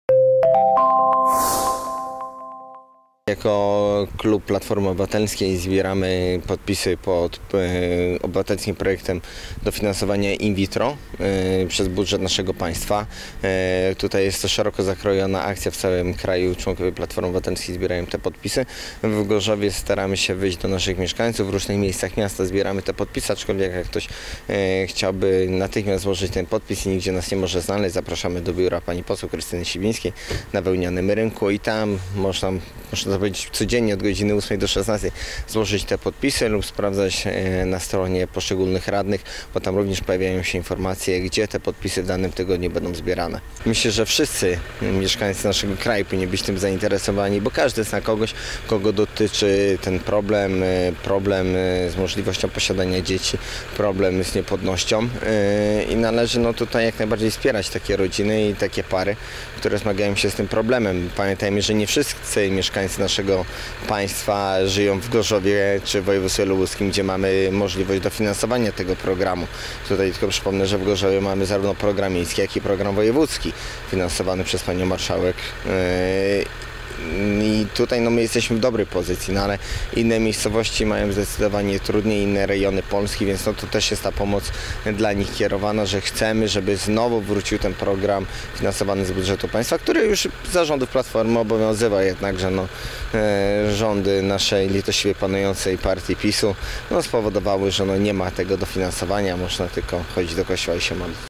Zbiórka podpisów o wsparcie programu in vitro
Radni i działacze Platformy Obywatelskiej przypominają o trwającej zbiórce podpisów pod obywatelskim projektem dofinansowania in vitro. Projekt jest ogólnopolski, wzywający do pomocy przez polski rząd parom zmagającym się z niepłodnością . Aby złożyć podpis można skontaktować się z radnymi PO w Gorzowie lub odwiedzić biuro poselskie Krystyny Sibińskiej przy Wełnianym Rynku.